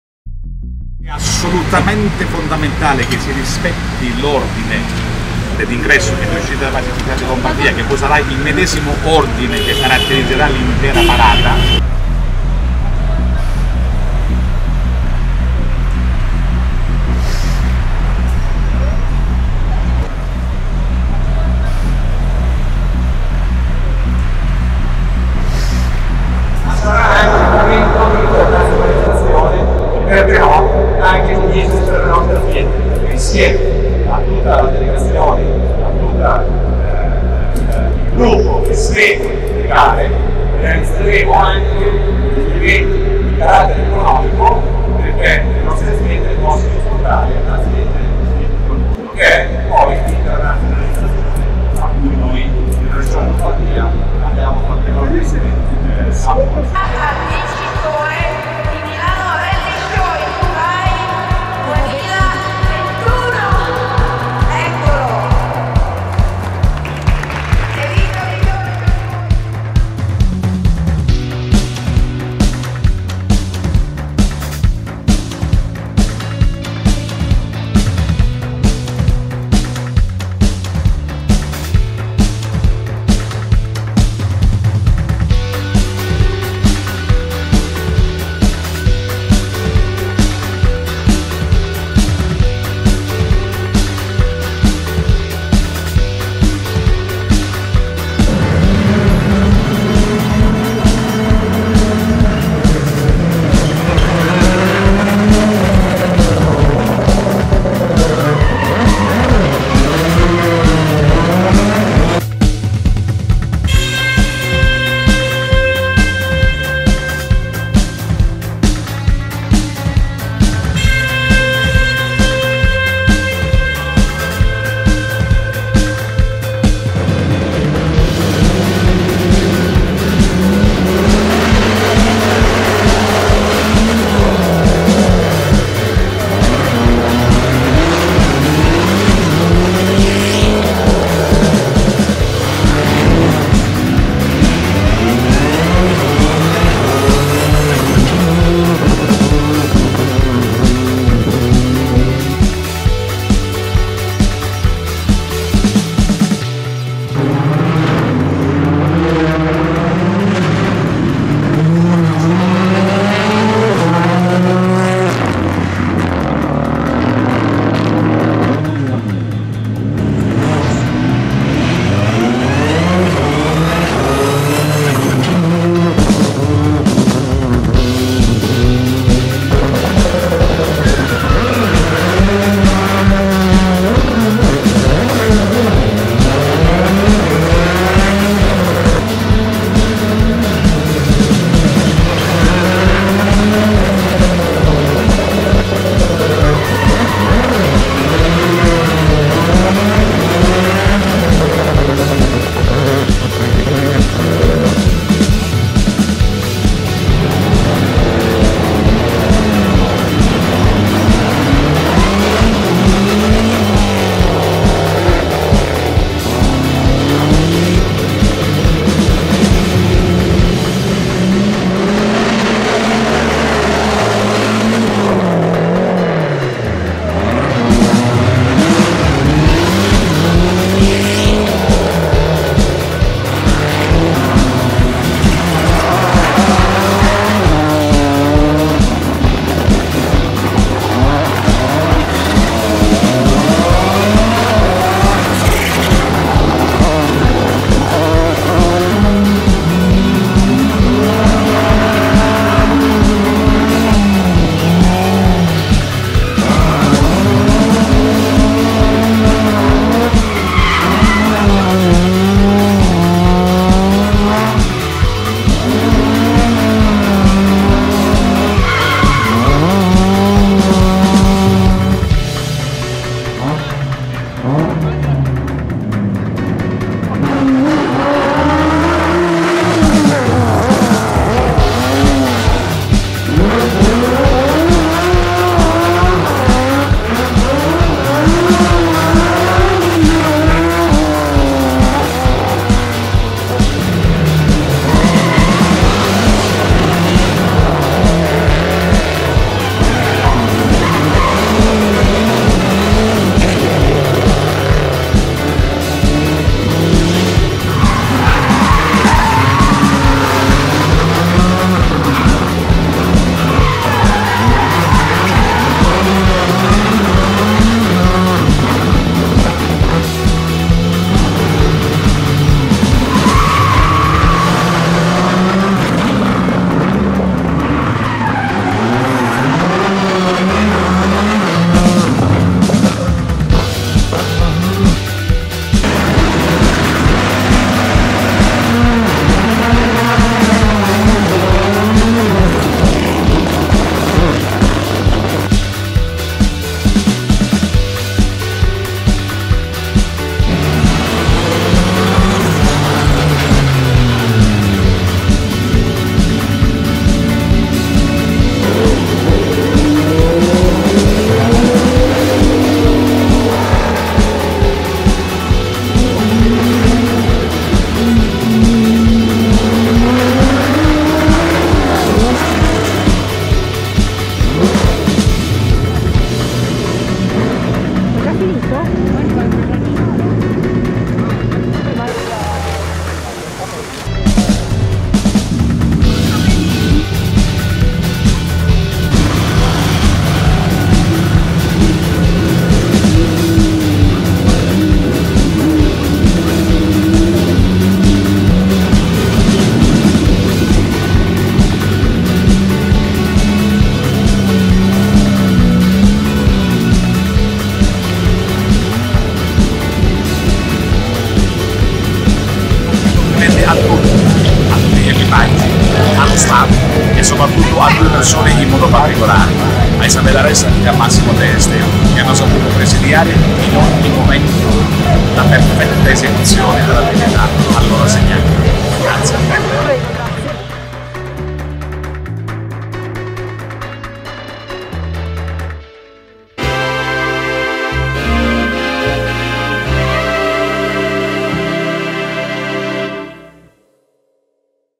"One year to go" Milano Rally Show Dubai
Milano è stata identificata come sede per ospitare uno degli eventi principali dell'evento One year to go, testimoniando un passaggio di consegne simbolico tra l'Expo 2015 e la prossima manifestazione di Dubai.